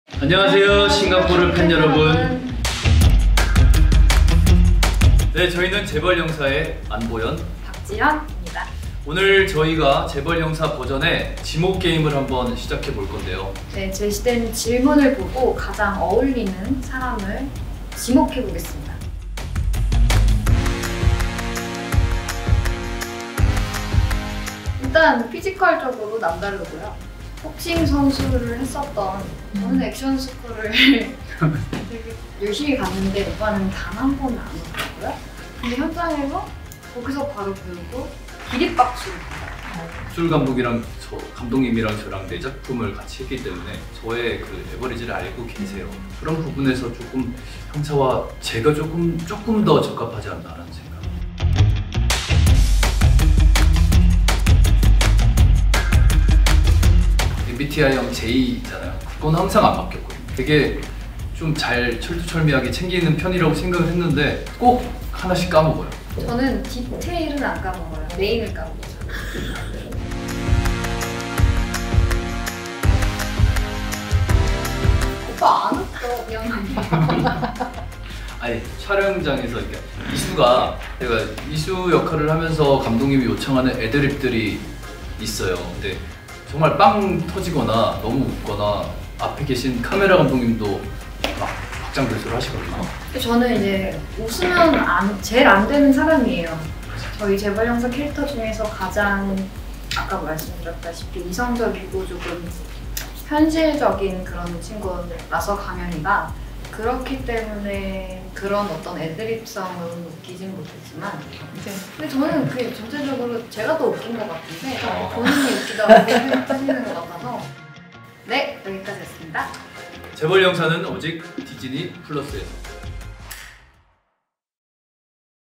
Who's Most Likely To | Flex x Cop | Disney+ Singapore
Are Ahn Bohyun and Park Jihyun anything like Isoo and Ganghyun IRL? 🤔

Get to know our charismatic leads as they play Who’s Most Likely To… and catch FlexxCop now on DisneyPlusSG! 🚨